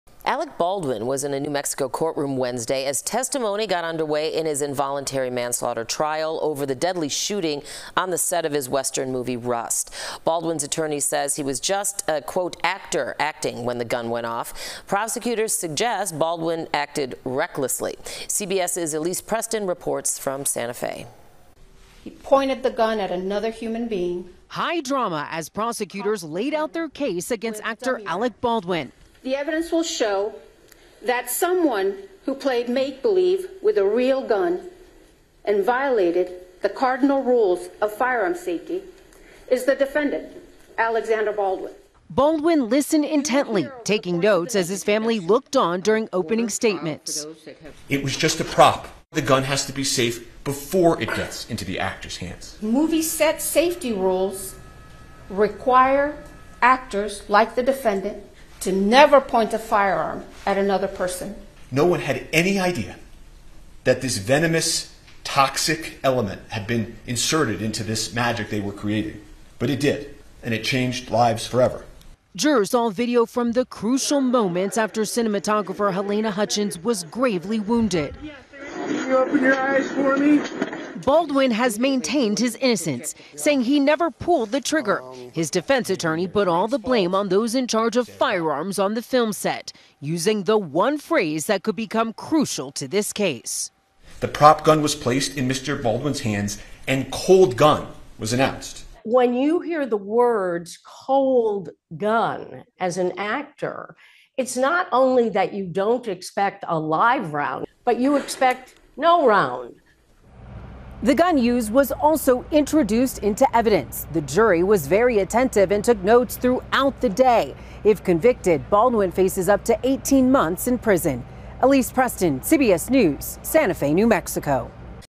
Witness testimony begins in Alec Baldwin manslaughter trial
Baldwin's attorney says he was just "an actor acting" when the gun went off. Prosecutors suggest Baldwin acted recklessly.